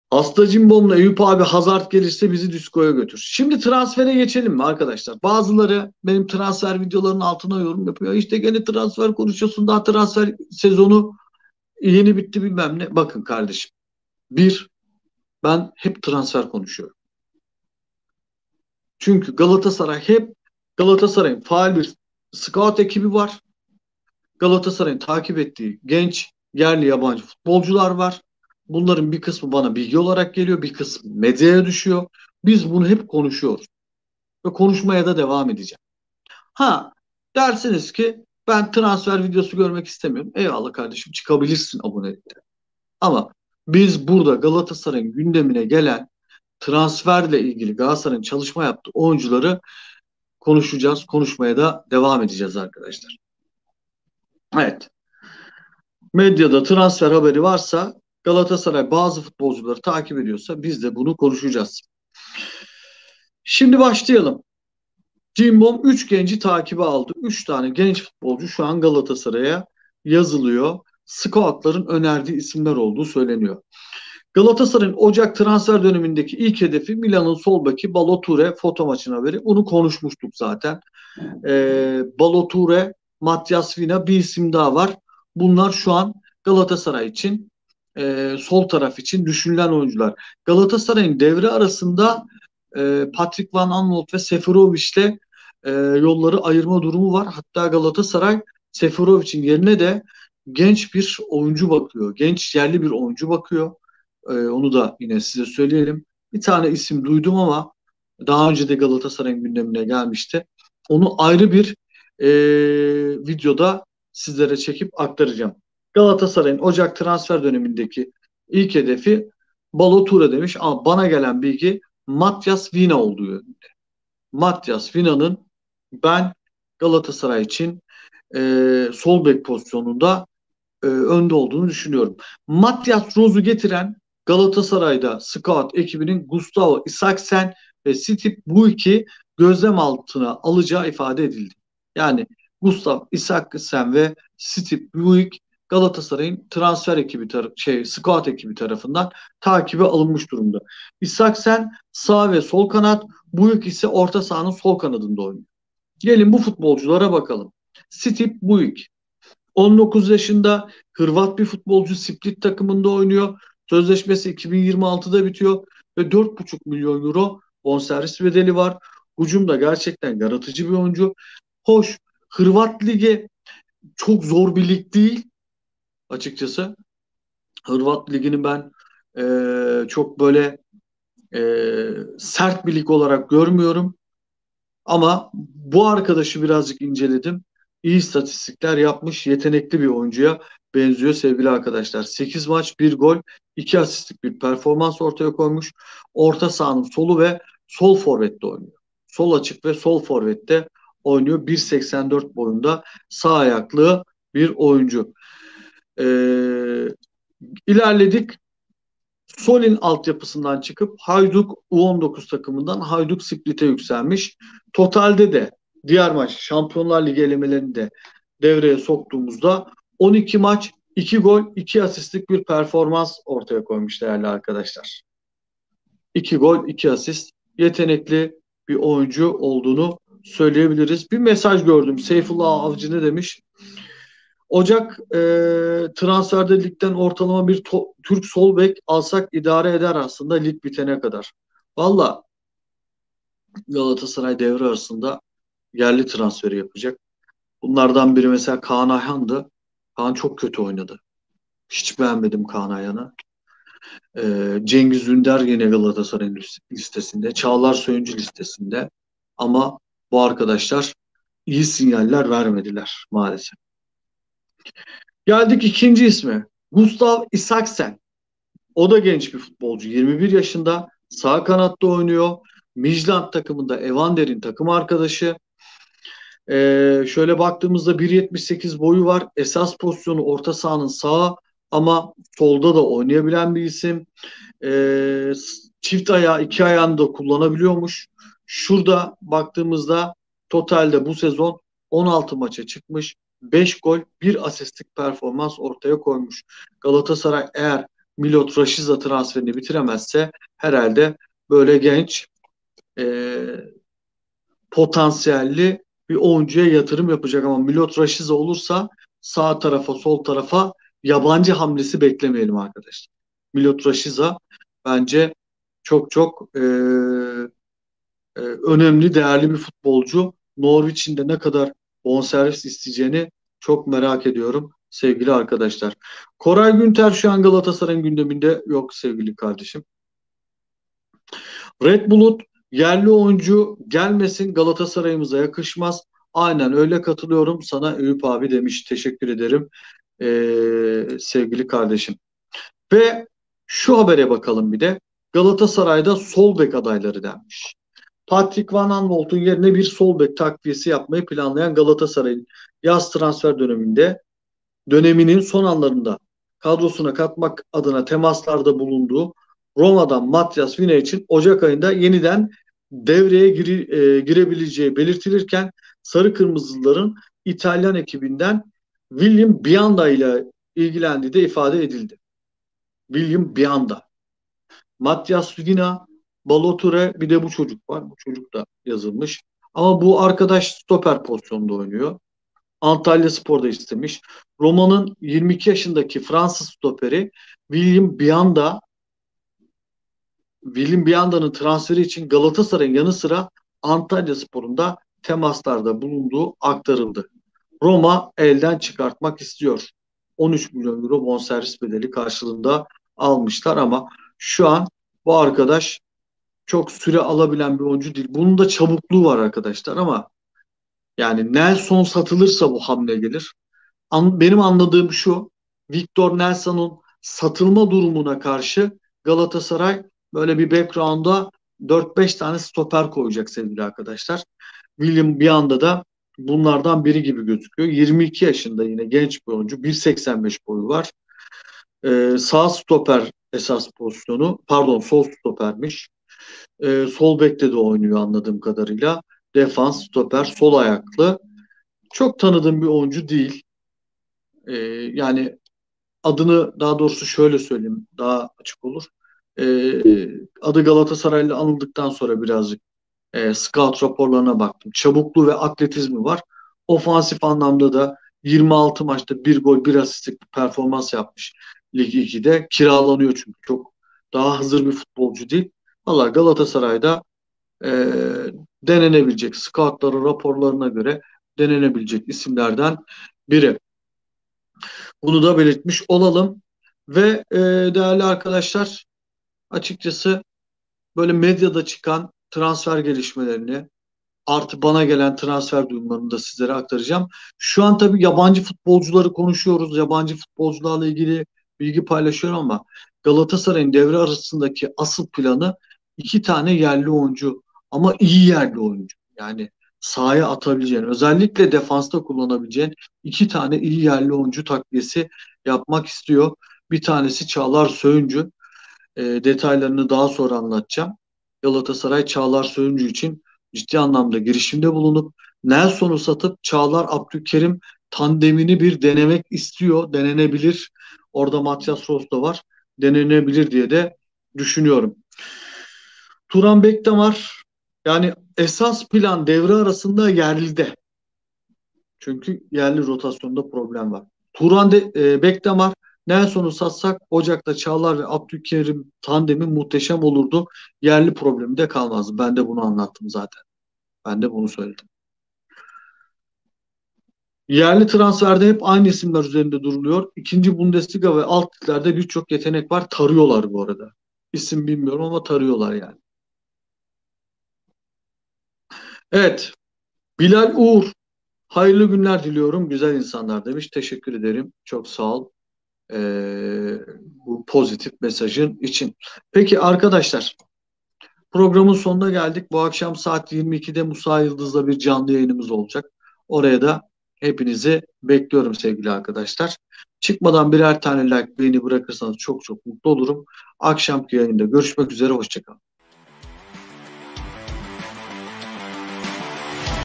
GALATASARAY'DA SCOUT EKİBİ 3 GENÇ OYUNCUYU TRANSFER ETMEK İÇİN TAKİBE ALDI | SOL BEK HAREKATI
Son dakika Galatasaray ve transfer haberleri. GS'de scout ekibi 3 genç oyuncuyu takibe aldı. Radyospor programcısı Eyüp Yıldız, Galatasaray'ın Süper Lig'de oynadığı ve oynayacağı maçlarını ve transferde yaşanan son gelişmeleri değerlendiriyor.
Galatasaray'dan haberler, Türk Sporundan yorumlar, futbol basketbol ve diğer branşlardan bilgiler ve yorumlar için kanalıma abone alabilirsiniz.